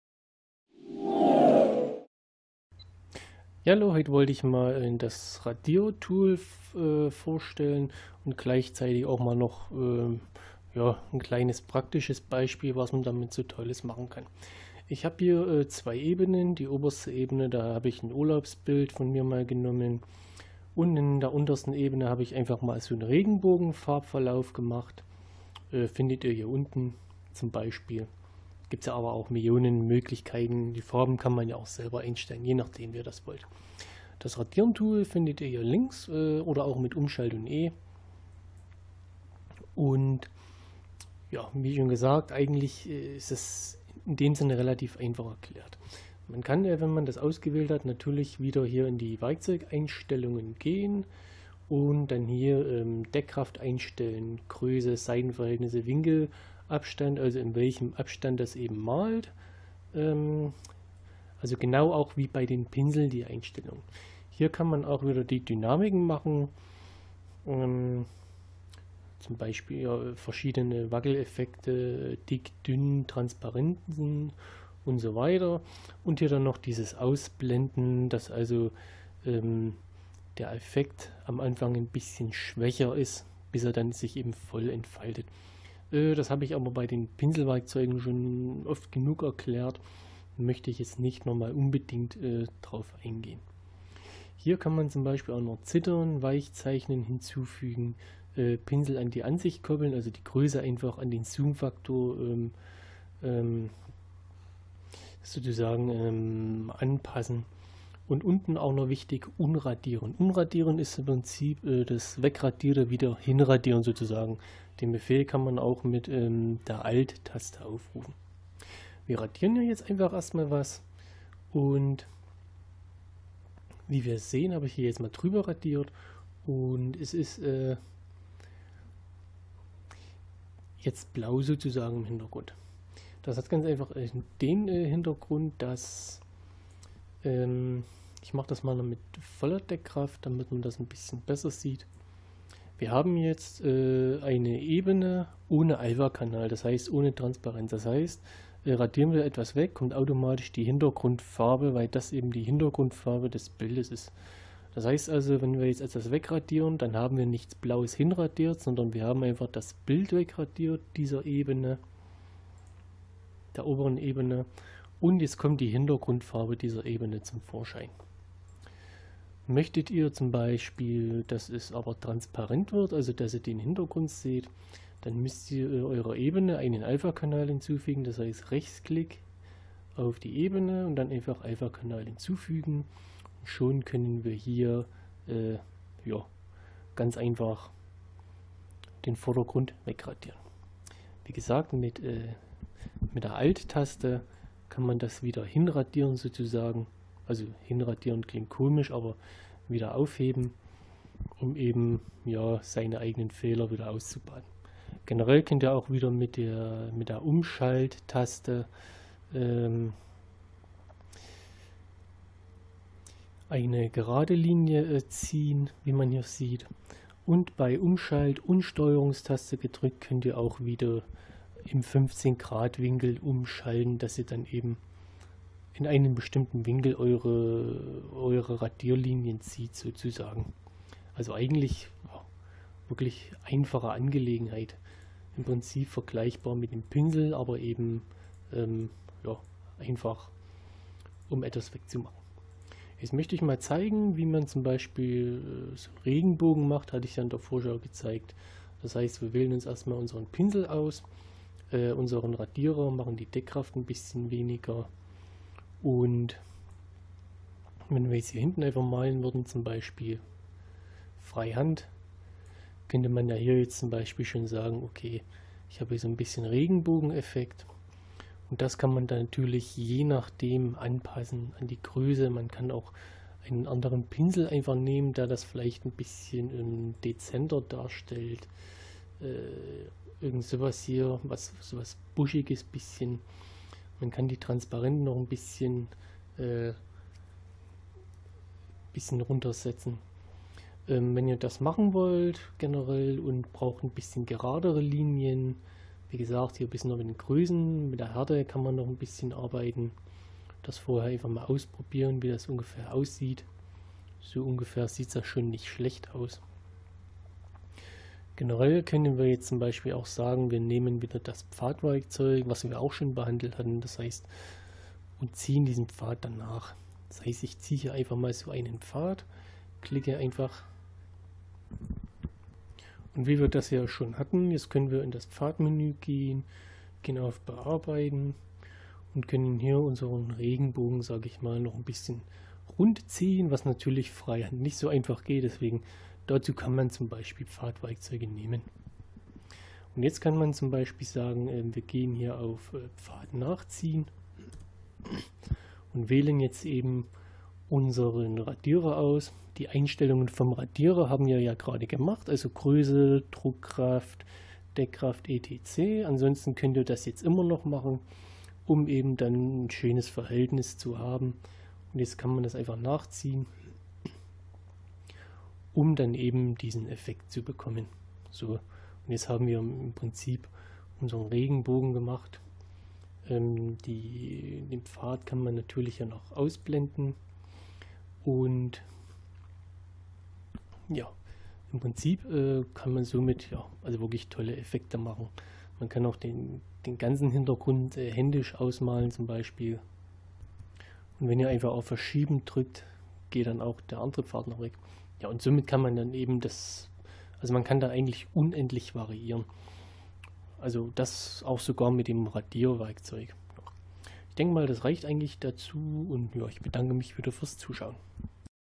Gimp 2022 #19 TOOL Radierer + Beispiel Regenbogen Effekt
Hier die Erklärung zum Tool Radierer, wie man ihn anwendet und ein tolles Beispiel wie man einen Regenbogen einfach erzeugen kann.